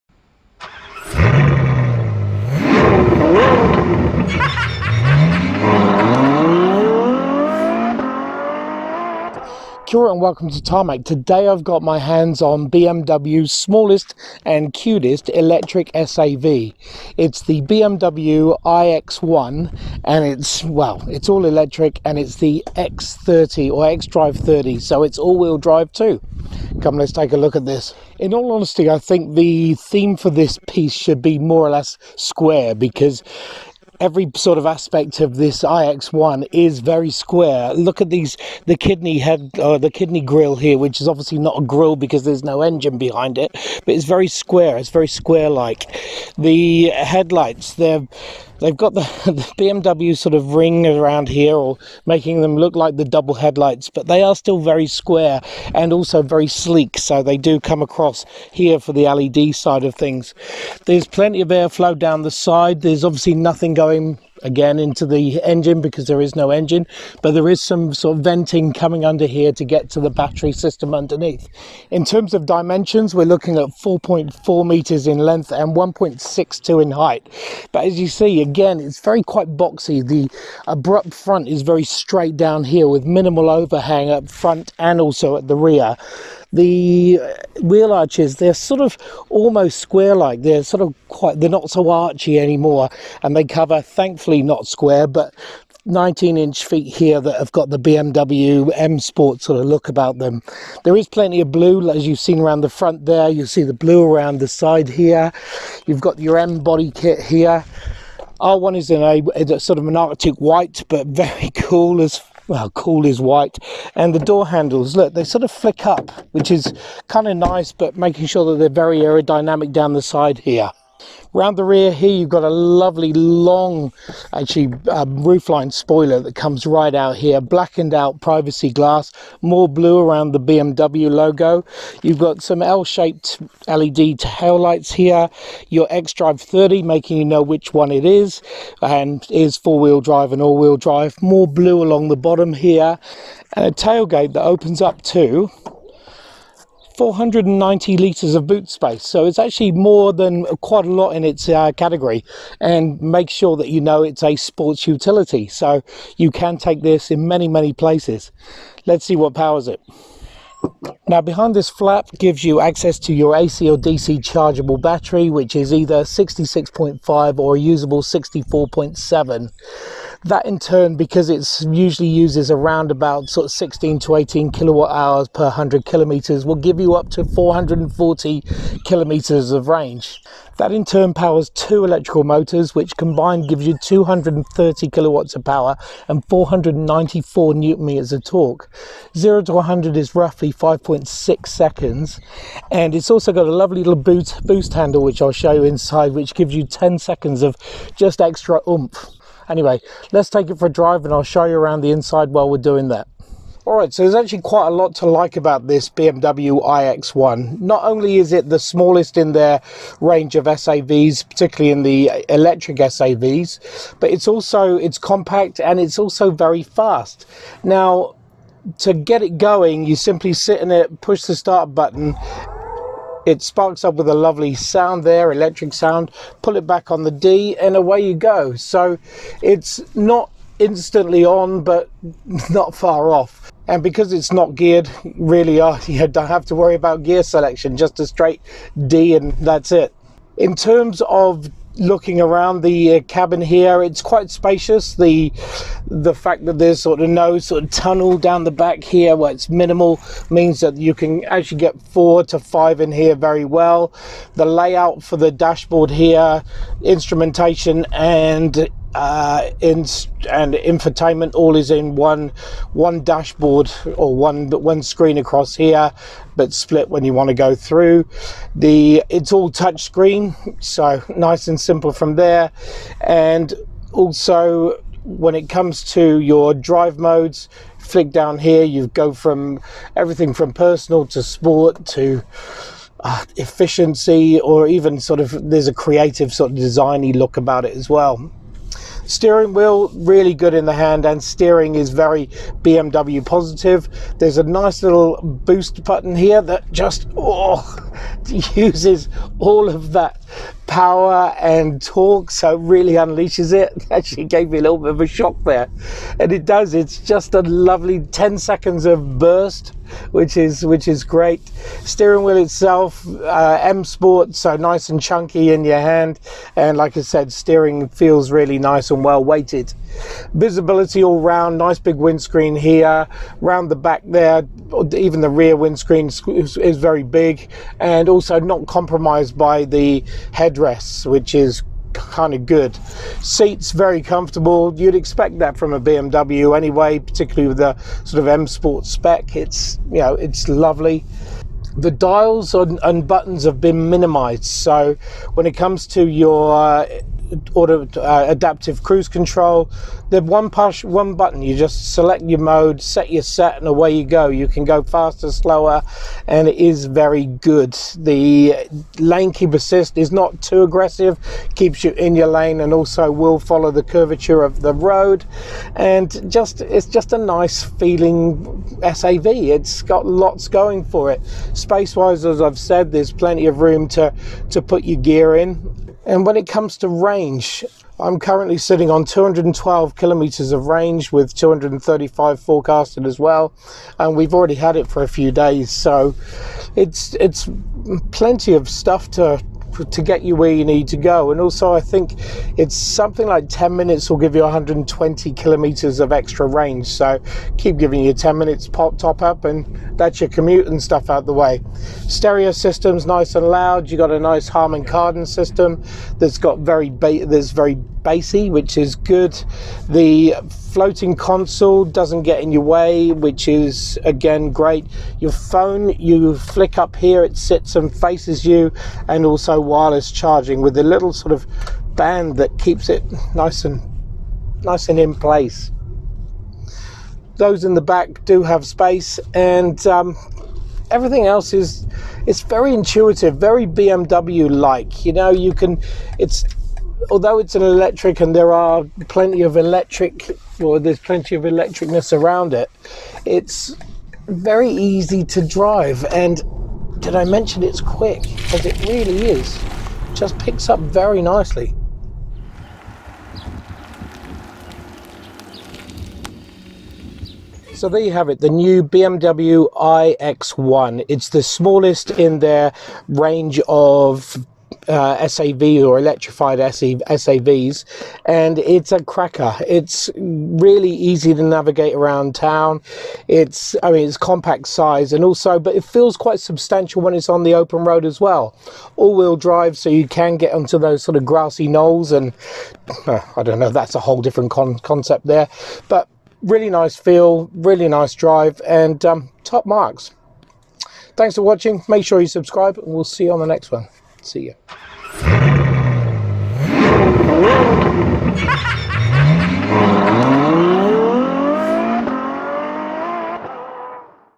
BMW iX1 xDrive30 review
We find out just how good BMW's new all-electric baby SAV is. Dual motors, heaps of range, quick off the mark and all-wheel drive. Easy to park but holds its own on the highway.
for more news and reviews.
Cheers for the opportunity BMW NZ